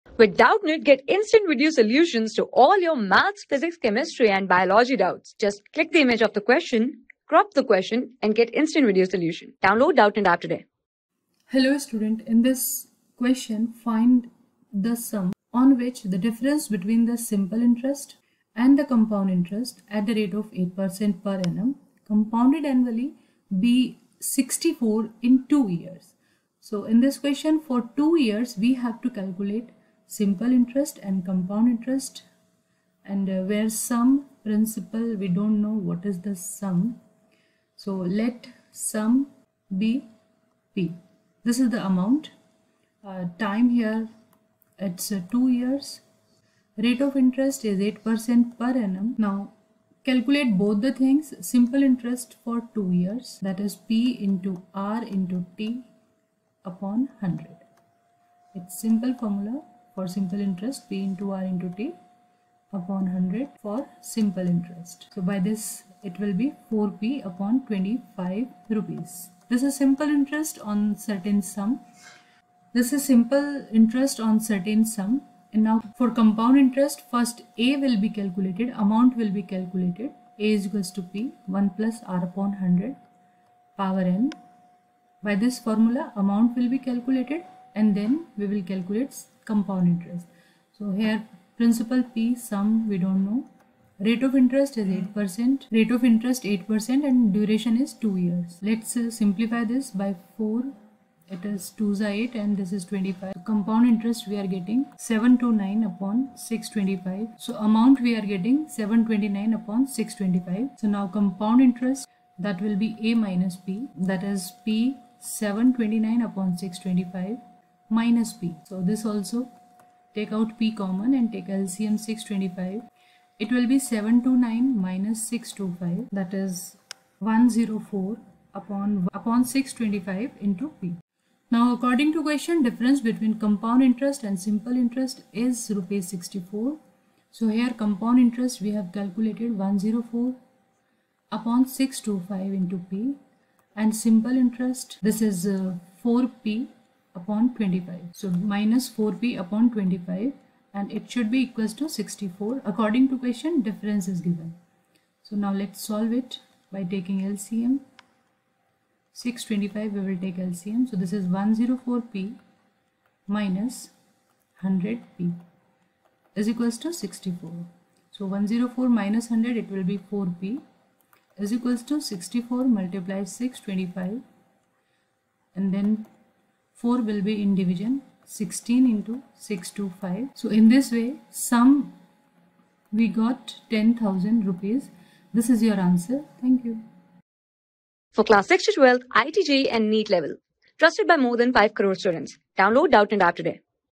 Find the sum on which the difference between the simple interest and the compound interest at th...
Find the sum on which the difference between the simple interest and the compound interest at the rate of 8% per annum compounded annually be रु 64 in 2 years .
Class: 9
Subject: MATHS
Chapter: COMPOUND INTEREST
Board:ICSE

👉 Have Any Query? Ask Us.